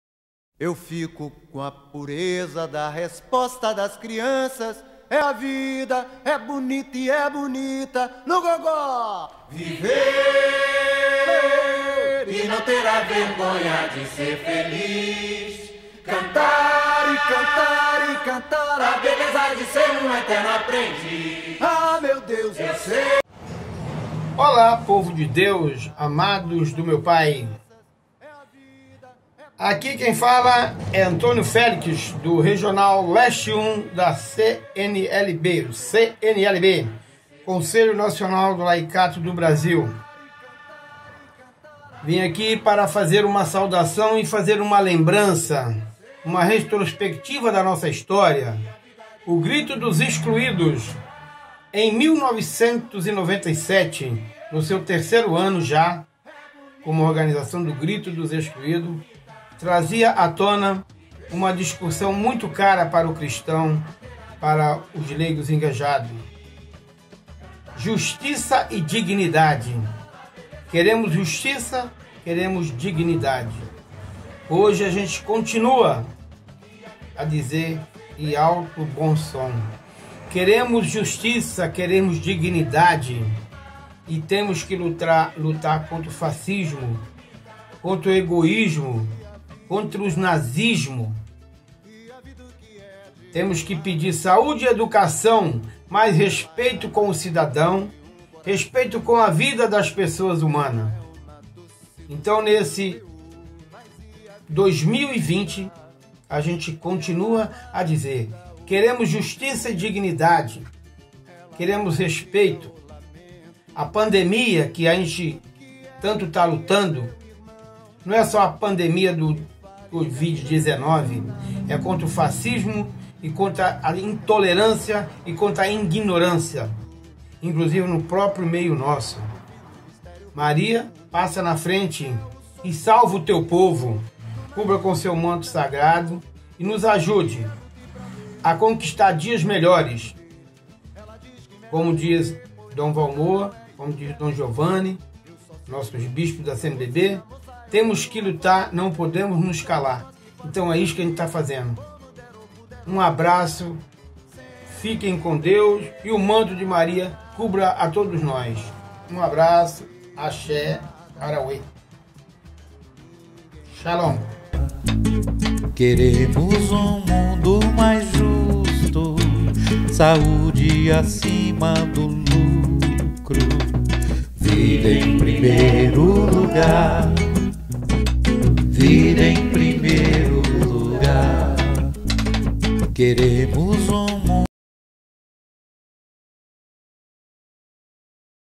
3º GRITO 1997 - 25 ANOS DE MEMÓRIA DO GRITO DOS EXCLUÍDOS
CNLB – CONSELHO NACIONAL DO LAICATO DO BRASIL

Colaboração: CNLB LESTE 1

25 ANOS DE MEMÓRIA DO GRITO DOS EXCLUÍDOS

 3º GRITO DOS EXCLUÍDOS 1997

O CNLB, antecedendo o 26º Grito dos excluídos, preparou juntamente com os Cristãos Leigos e Leigas de todo o Brasil, um histórico dos vinte e cinco gritos que estiveram presentes em nossa Igreja e sociedade.
Tendo como sempre o GRITO: VIDA EM PRIMEIRO LUGAR!

A Criatividade, a Metodologia e o Protagonismo dos Excluídos são marcas do Grito, que privilegia a participação ampla, aberta e plural. Os mais diferentes atores e sujeitos sociais se unem numa causa comum, sem deixar de lado sua especificidade.

Hoje, queremos fazer memória ao 3º Grito dos Excluídos/as, realizado em 1997 e que teve como lema: “Queremos justiça e dignidade”. O tema escolhido ampliou o debate trazido pela Campanha da Fraternidade daquele ano, que travava da questão dos encarcerados. O Grito aconteceu em mais de 700 cidades, mostrando a ampliação do alcance da iniciativa, ano após ano.

A reflexão trazida pelo Grito foi no sentido de questionar o modelo de justiça punitivo adotado no caso brasileiro, onde se aplica aqui medidas de encarceramento em massa e violência física aos condenados através das práticas de tortura e maus tratos no cárcere. É fato que esse é um modelo que não funciona e o que se observa como resultado dessa política é o aumento da população carcerária ao longo dos anos, deixando a solução dos problemas, cada vez mais distante.

Para esse ano, o Grito que quero dar é pelo direito a um sistema de justiça que de fato seja justo, já que não são raros os casos de privilégios concedidos a aqueles que cometem crimes, mas por possuir uma condição financeira melhor, acabam beneficiados por outras formas mais leves de cumprimento de pena.
Defender o direito a uma Justiça isenta e sem privilégios é nossa obrigação como Cristão leigo e leiga.